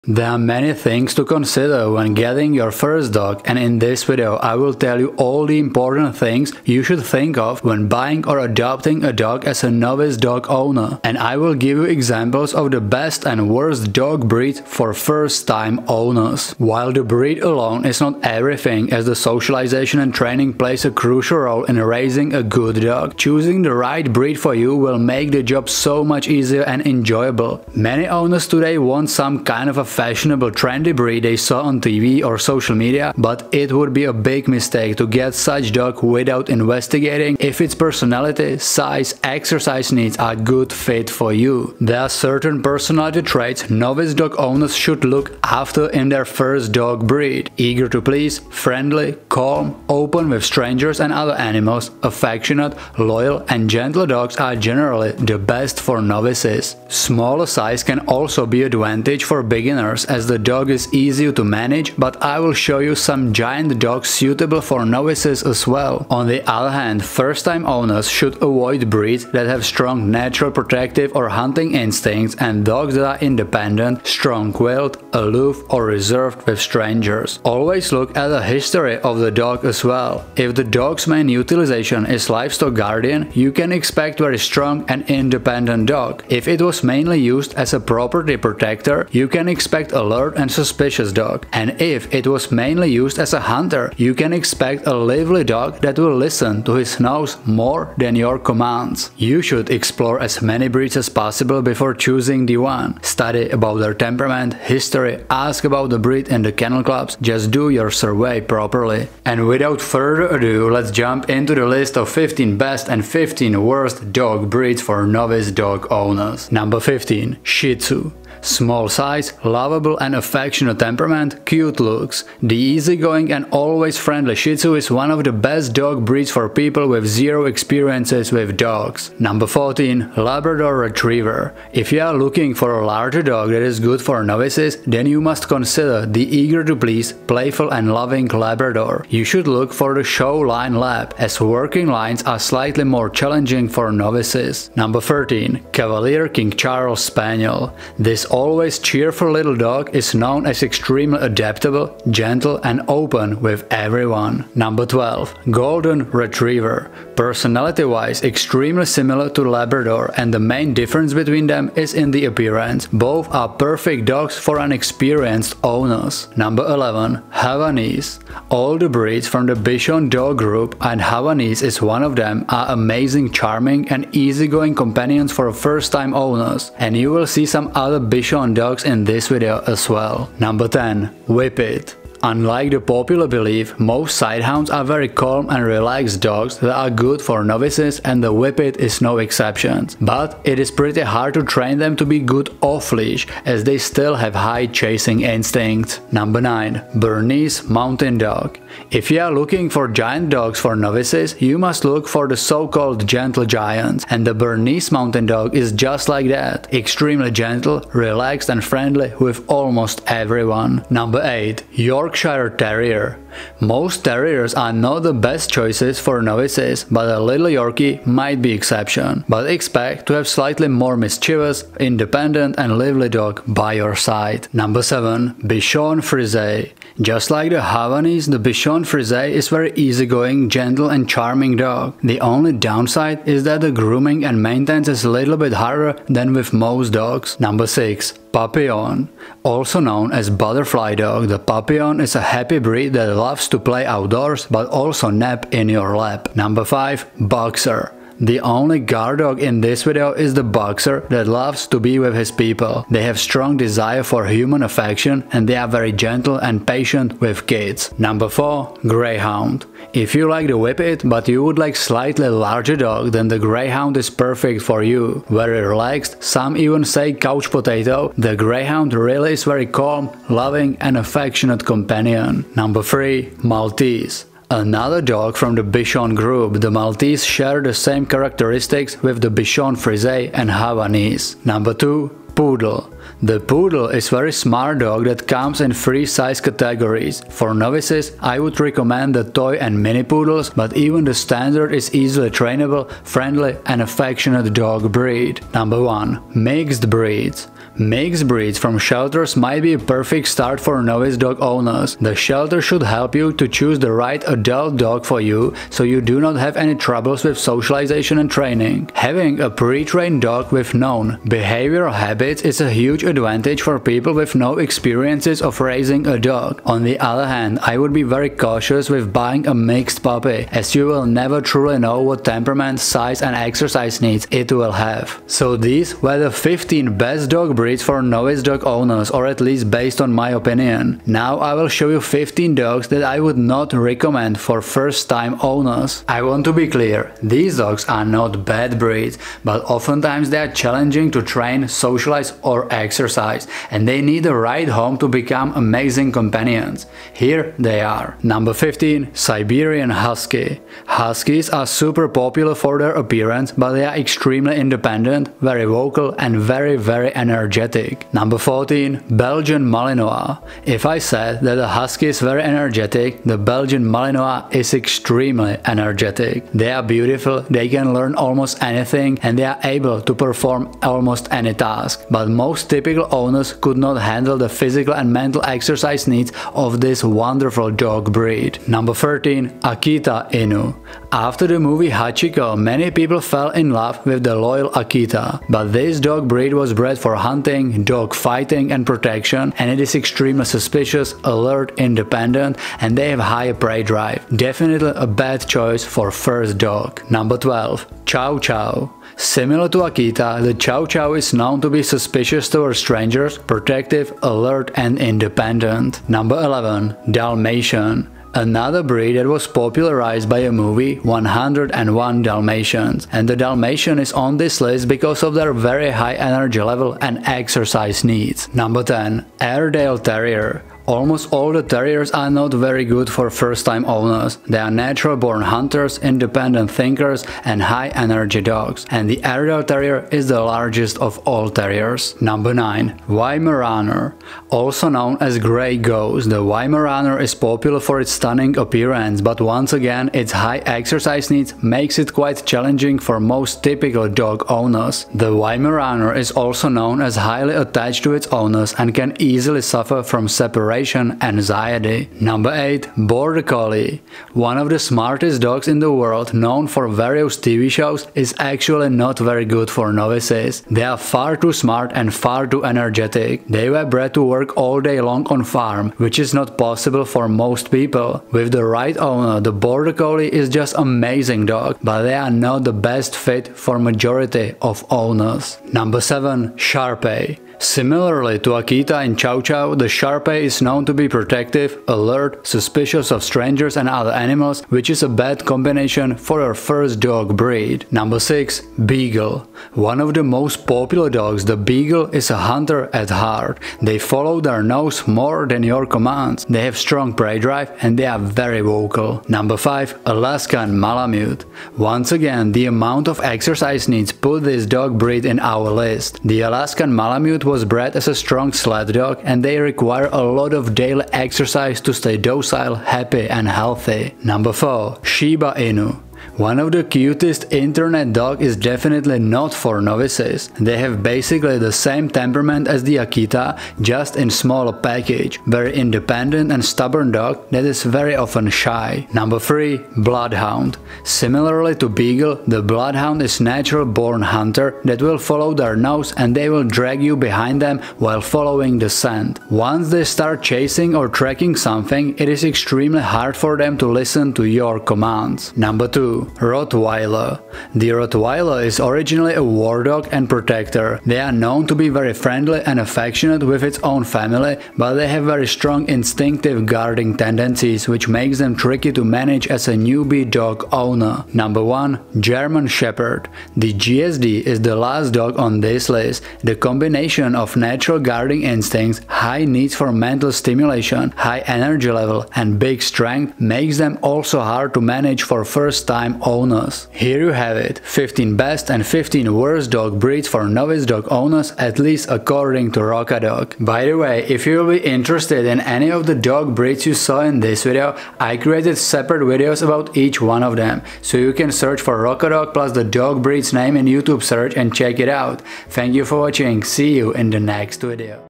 15 Best and 15 Worst Dog Breeds For First-Time Dog Owners
Best and worst dog breeds for novice and first time dog owners. Certain dog breeds are much better for people with zero experiences with owning a dog. Best dog breeds for novices are eager to please, friendly and affectionate. Worst dog breeds for first time dog owners are independent, aloof and shy. In this video i will show you 15 best and 15 worst dog breeds for novices.

Best and worst dog breeds for novice dog owners TIMESTAMPS:
0:00 What to consider when getting a new dog
1:57 Best dog breeds for first time dog owners
5:48 Worst dog breeds for first time dog owners